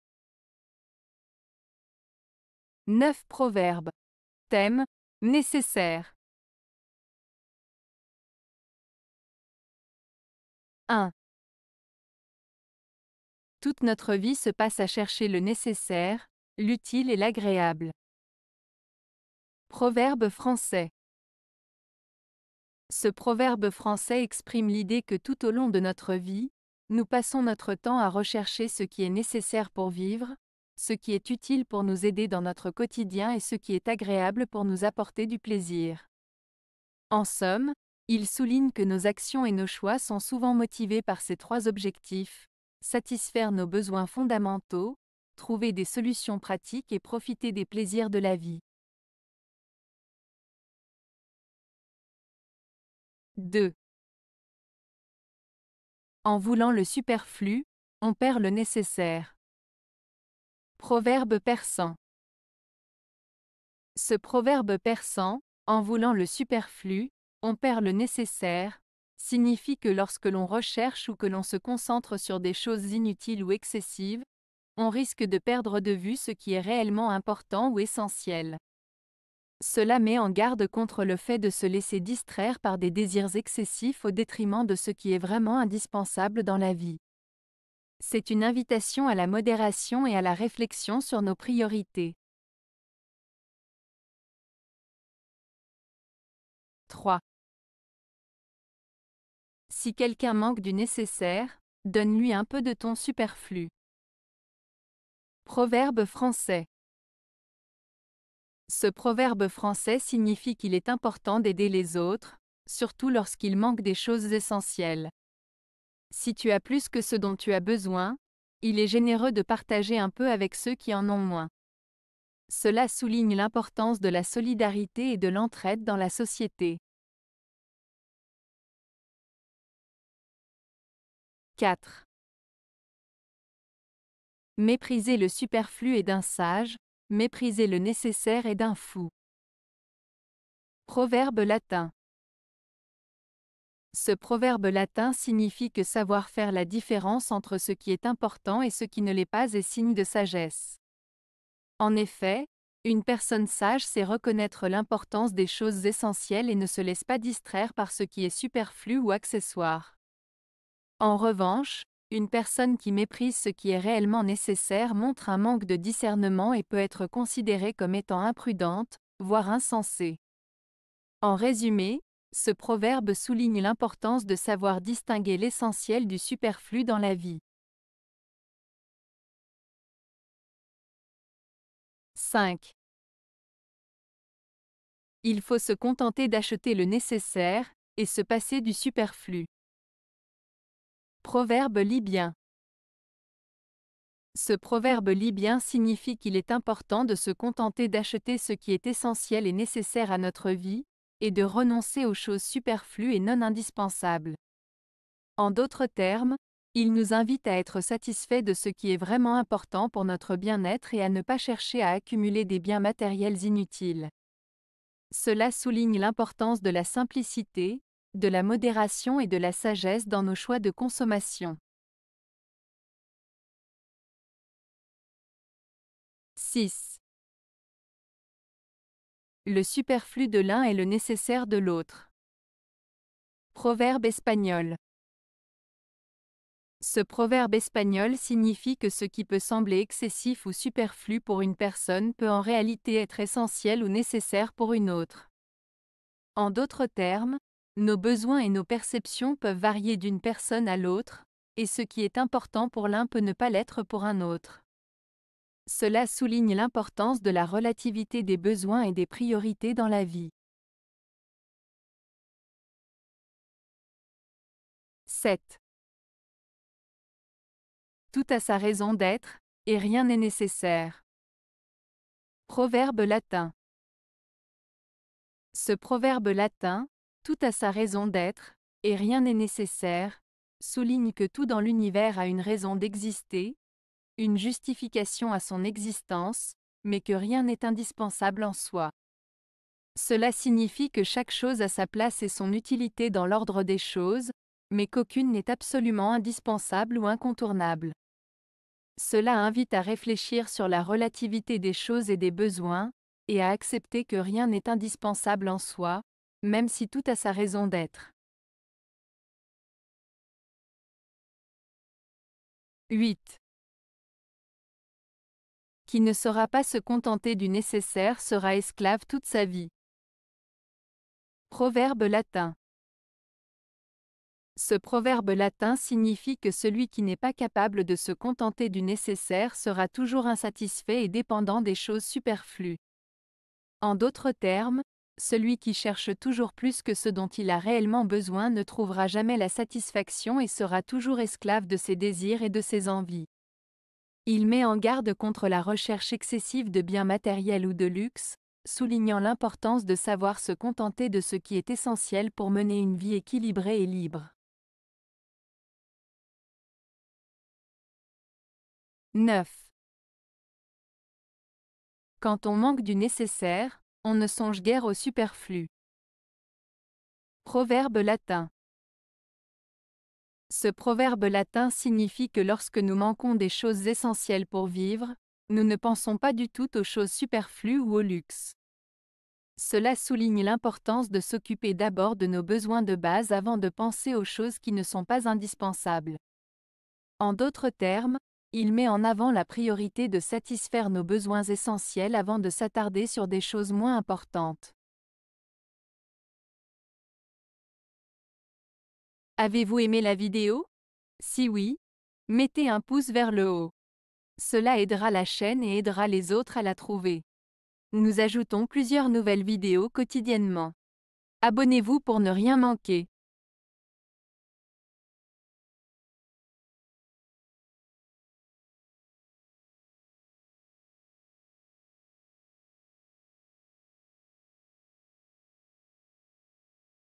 Proverbes - Nécessaire (3 de 3)
00:00 Introduction
00:08 Proverbe Français - Toute notre vie se passe à chercher le nécessaire, l'utile et l'agréable.
00:50 Proverbe Persan - En voulant le superflu, on perd le nécessaire.
01:34 Proverbe Français - Si quelqu'un manque du nécessaire, donne-lui un peu de ton superflu.
02:09 Proverbe Latin - Mépriser le superflu est d'un sage, mépriser le nécessaire est d'un fou.
02:59 Proverbe Libyen - Il faut se contenter d'acheter le nécessaire, et se passer du superflu.
03:43 Proverbe Espagnol - Le superflu de l'un est le nécessaire de l'autre.
04:22 Proverbe Latin - Tout a sa raison d'être, et rien n'est nécessaire.
05:10 Proverbe Latin - Qui ne saura pas se contenter du nécessaire sera esclave toute sa vie.
05:56 Proverbe Latin - Quand on manque du nécessaire, on ne songe guère au superflu.